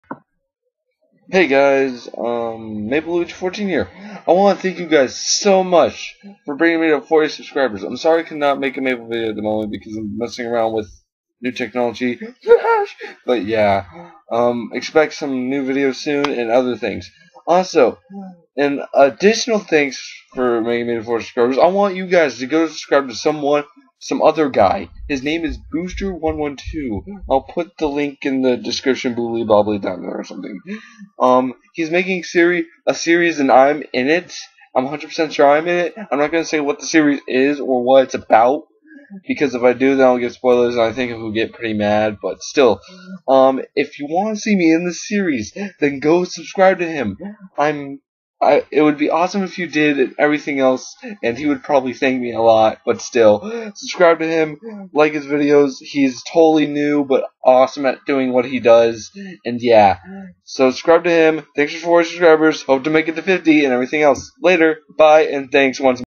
40 subz
Thx all for 40 subz i love you all! *in a fanbase kinda way*
like i said the guy you sould sub to is rigth there sub him
*sorry for messing up the user name*